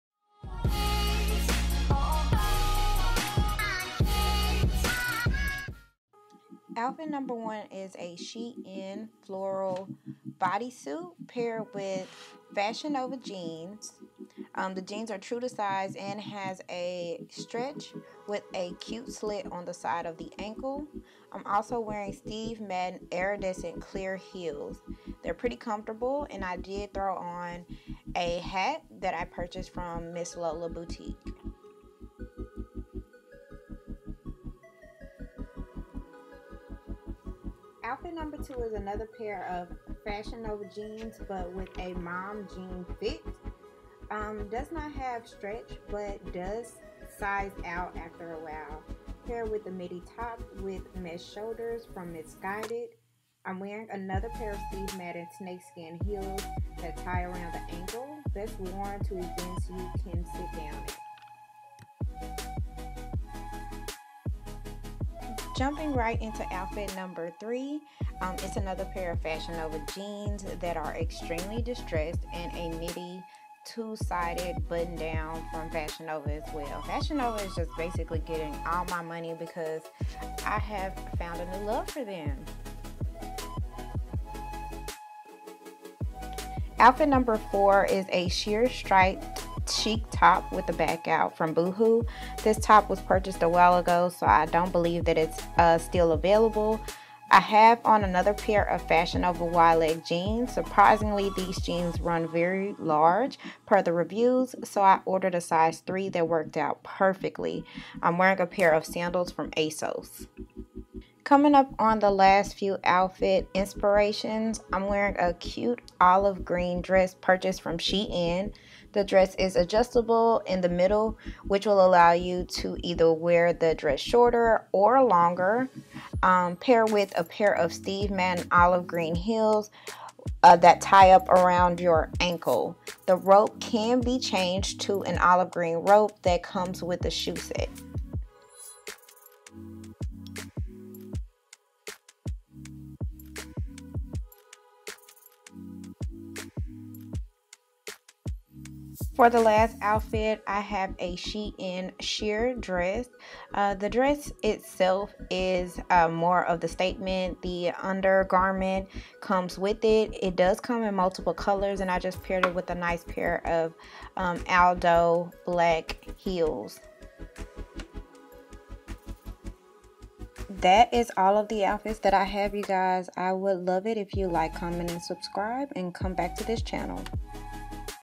Spring Essentials Try on Haul
Although we're all sitting in the house at this point in time doesn't mean we can't take the steps we need to get ready for the outdoors. I've included several Spring Inspiration Ideas and the link to each piece. Some pieces were already sold out but I have linked another option that match that style. Also invite others to my channel so that we can all become a big fashion forward family.

outfit #2
Top: Sold out

Outfit #4
Top : Sold Out